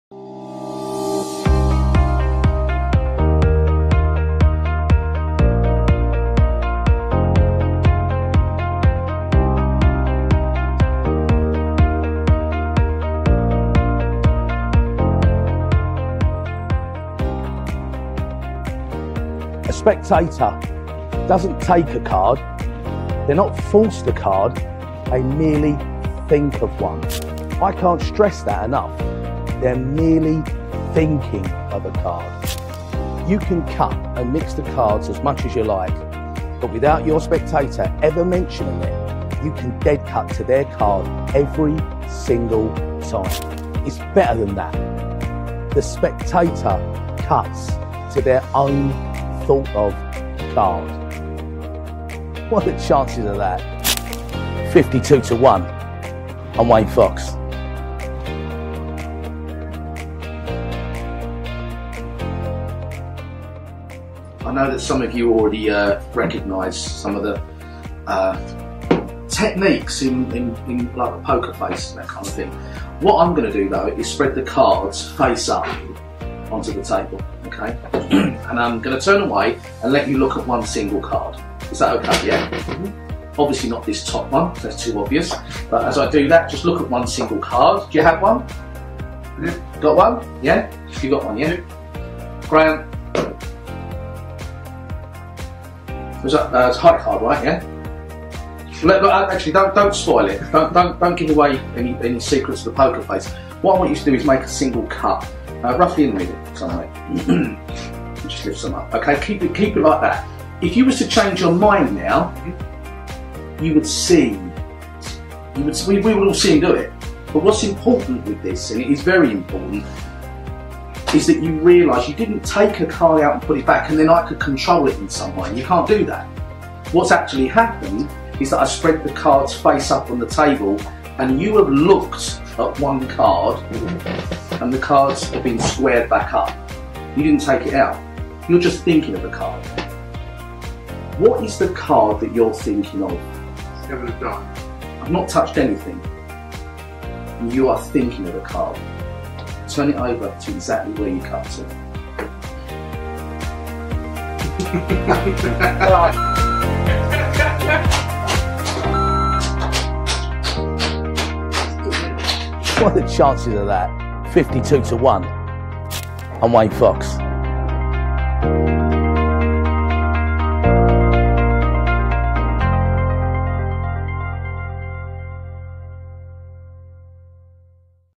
5221 by Wayne Fox
Available from your favorite magic dealer.

You ribbon spread or fan the deck and invite your spectators to think of any card.

You place the deck on the table and they name their card.

You then dead cut to the card perfectly!!!

In a follow up, the spectator can even cut to their own thought of card.

There is no wonder that Wayne Fox completely sold out of 5221 (Fifty Two To One) at the Blackpool convention.